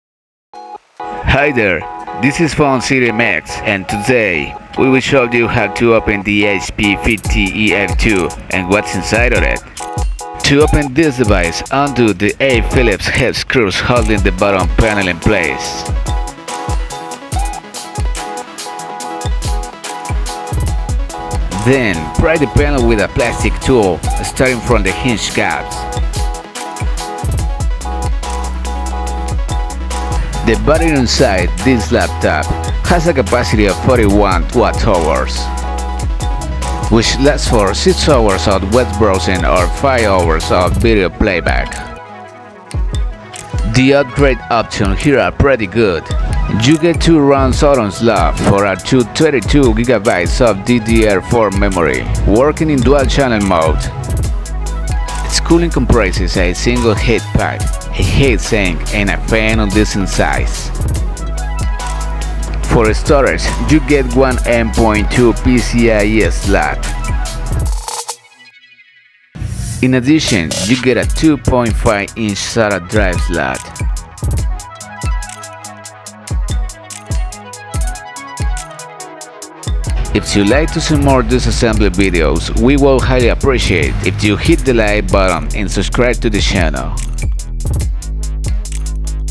🛠 How to Open HP 15-EF2 DISASSEMBLY and Upgrade Options ✅️
The flex to connect 2.5 inch SATA drive will need to be purchased separately.
This Laptop model is oriented for web browsing and non-demanding graphic design.
•GPU: AMD RADEON GRAPHICS 512MB
•DRAM: DDR4 MAX BANDWIDTH 3200Mhz
•CPU: AMD RYZEN 5 (5500 U)